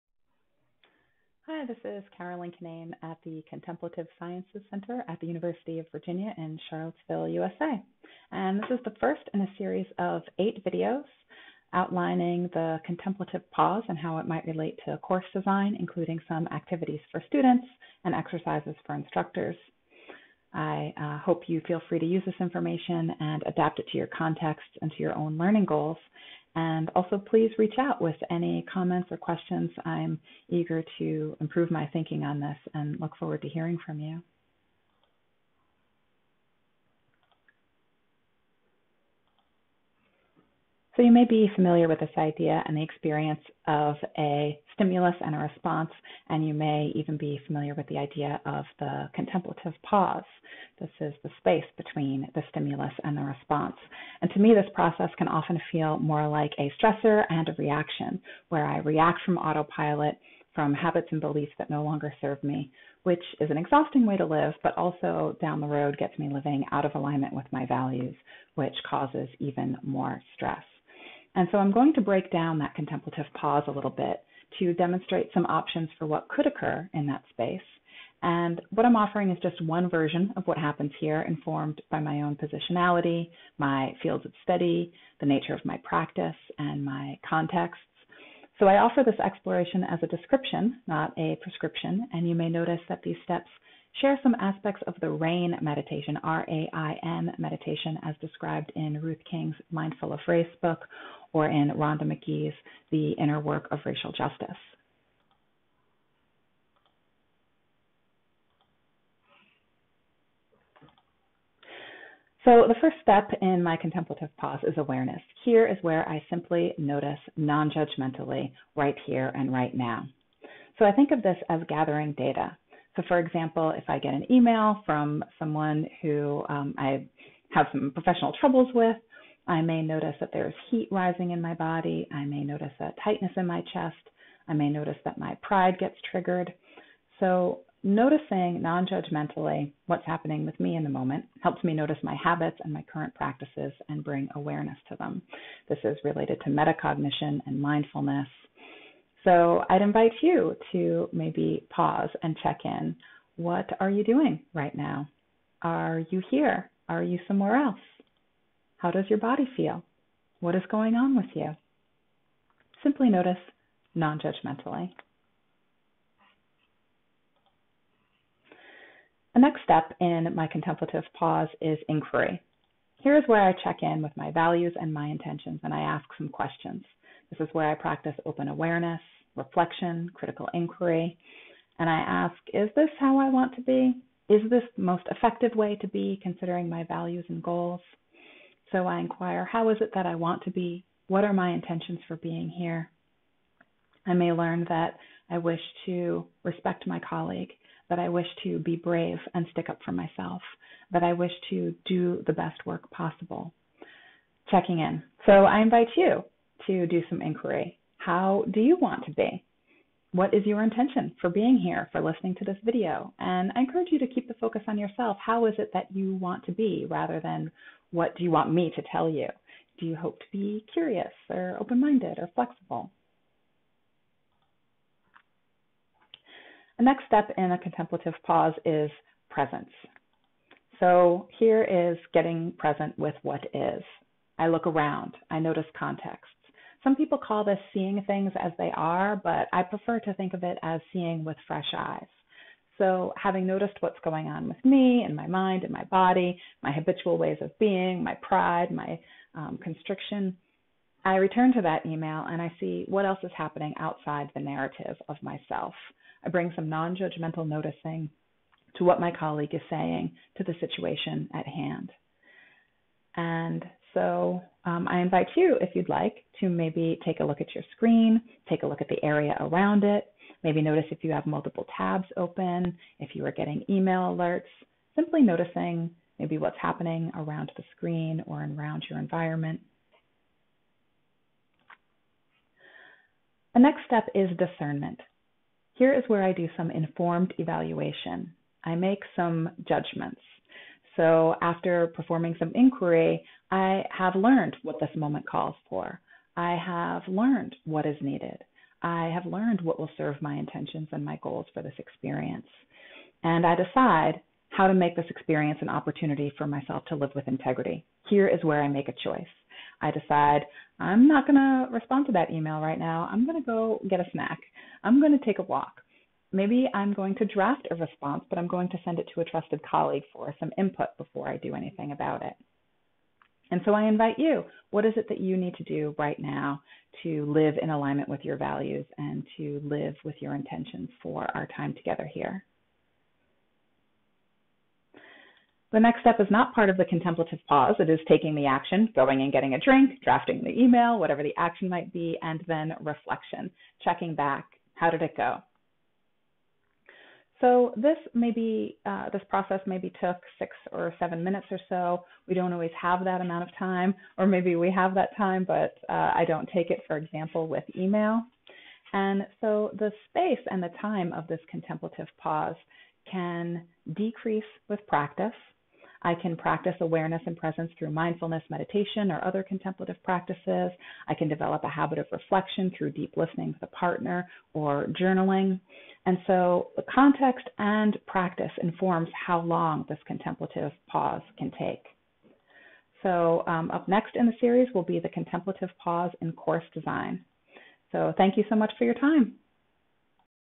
1. The Contemplative Pause
First in a series of eight videos. An overview of a contemplative process between stimulus and response featuring awareness, inquiry, presence, discernment, action, reflection. (6:30) To access the full series,